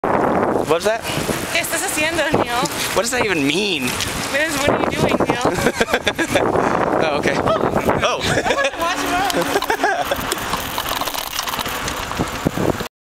Stefanie on her bicycle
Stefanie riding her bicycle and speaking spanish.

Videographer: Neil Atchley